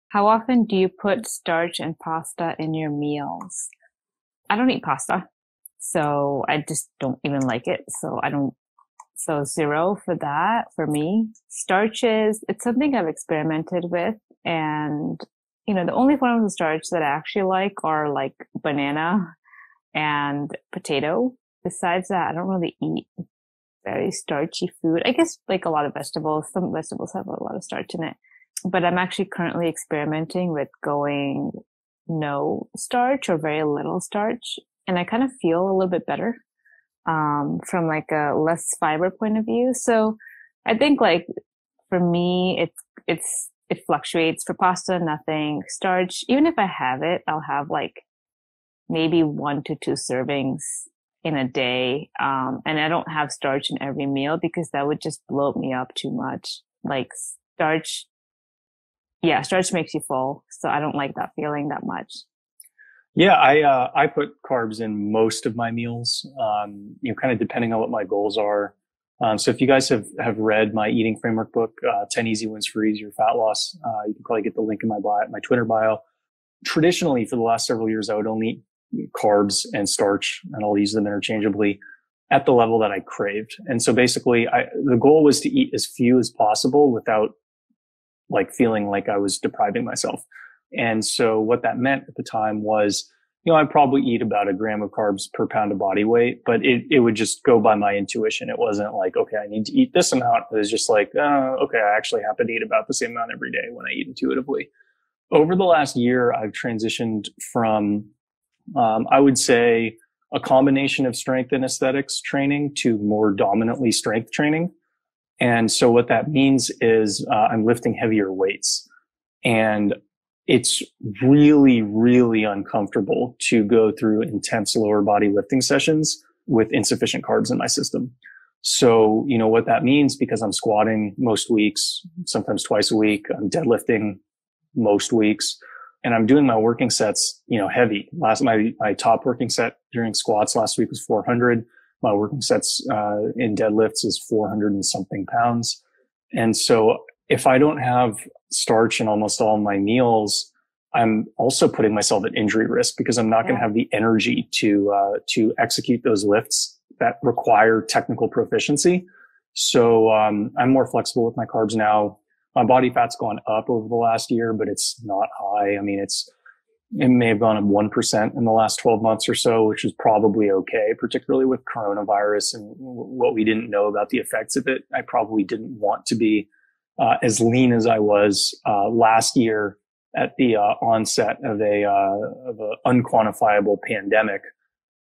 What Carbs Help You Ditch a Dad Bod
What is the optimal carb consumption for losing your dad bod?

Catch Preethi share her stance